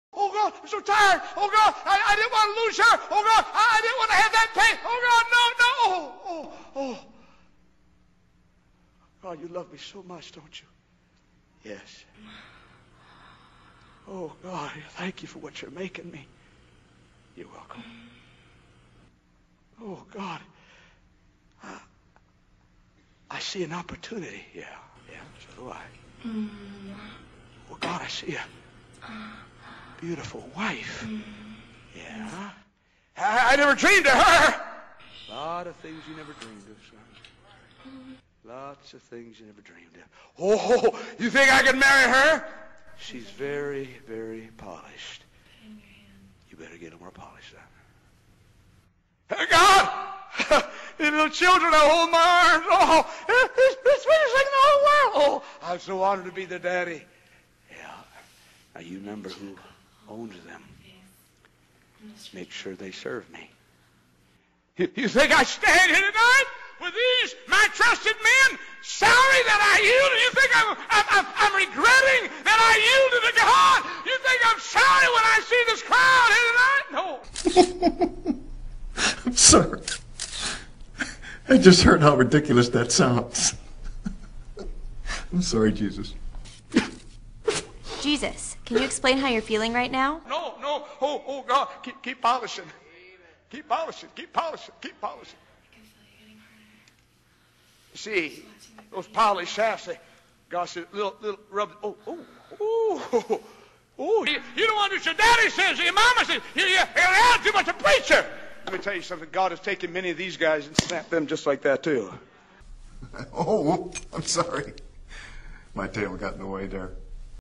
Pastor Jack Mehoff: polish to knob, spoil the child
wanking for jebus, Dr. Jack Schaap Preaches at Youth Conference 2010